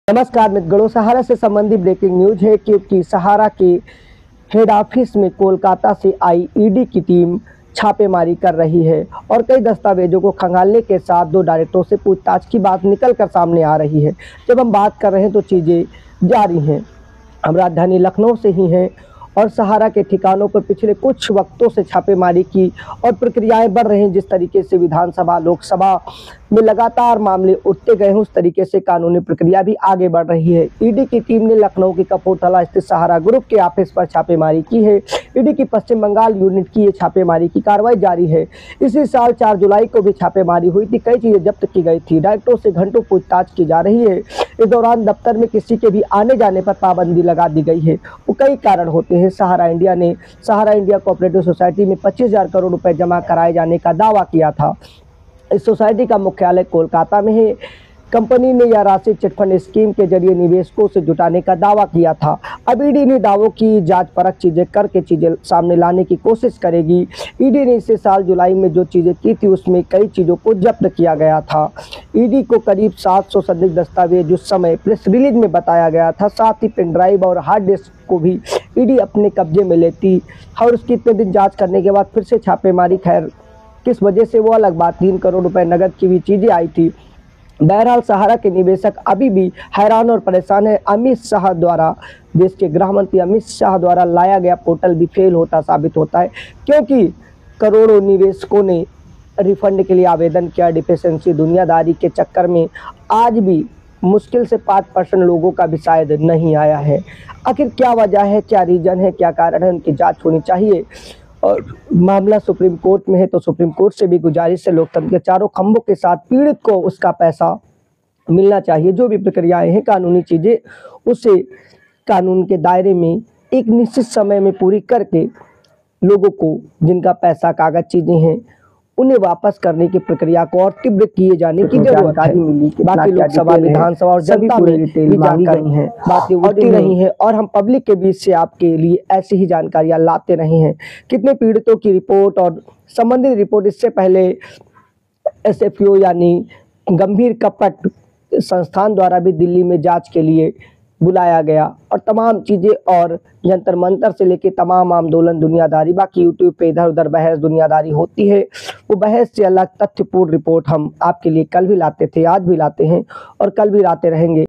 लखनऊ में ED की सहारा इंडिया परिवार के दफ्तर पर रेड, सहारा का क्या है पूरा मामला
कोलकाता चिट फंड केस: लखनऊ में सहारा ग्रुप के दफ्तरों में ED की छापेमारी, डायरेक्टरों से हुई पूछताछलखनऊ सहारा ग्रुप के ऑफिस में ED ने की छापेमारी, दिल्ली के भी कई ठिकानों पर रेड
प्रवर्तन निदेशालय की टीम ने लखनऊ सहारा ग्रुप के ऑफिस में छापेमारी की है। ईडी की टीम ने कल दिल्ली में भी सहारा ग्रुप से जुड़े कई ठिकानों पर छापेमारी की थी।